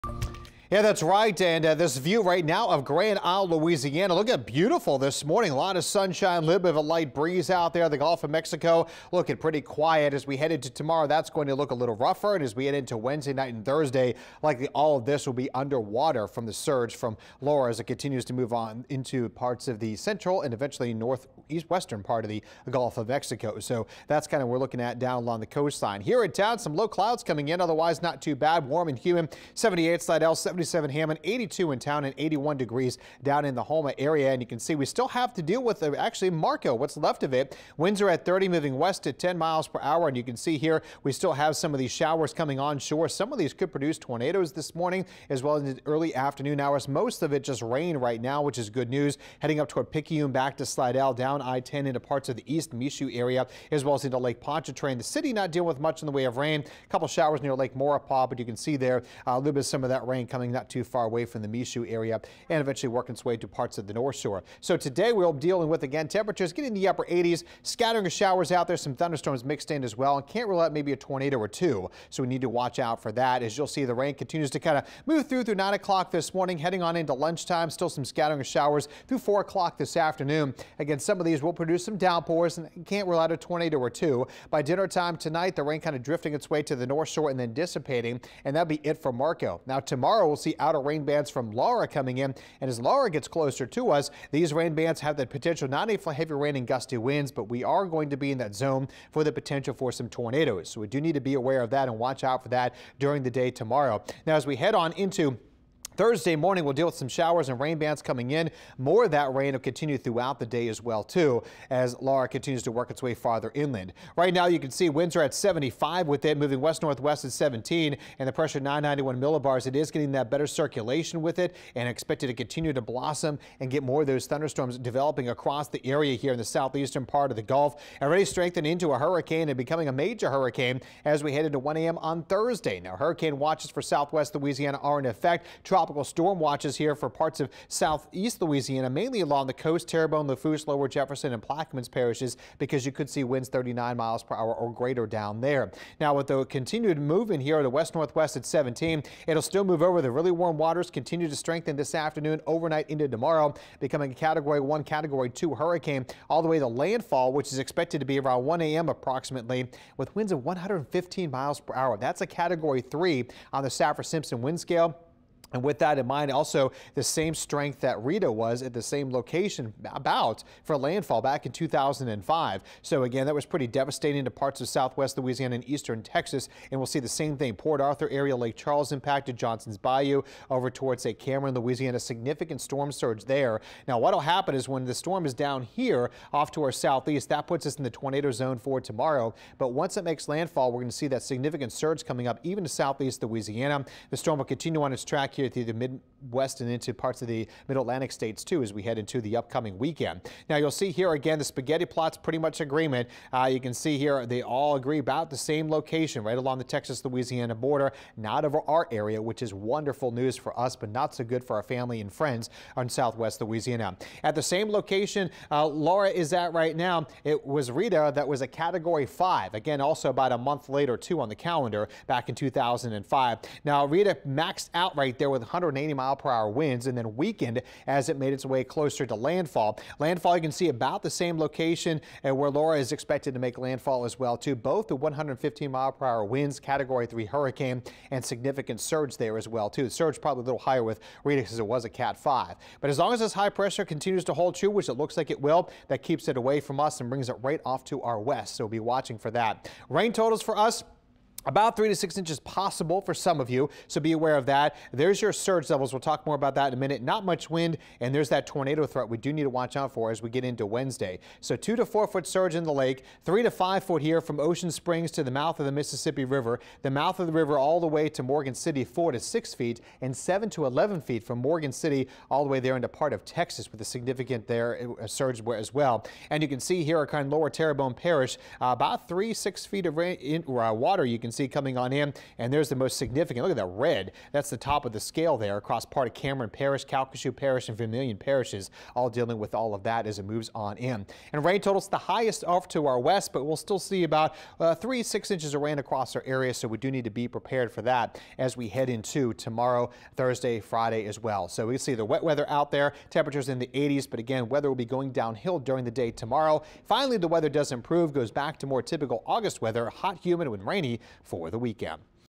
The Latest on Hurricane Laura and the Remnants of Marco
Meteorologist Dave Nussbaum says the remnants of Marco bring us rain and a few tornadoes today. Then we watch Hurricane Laura with impacts starting on Wednesday.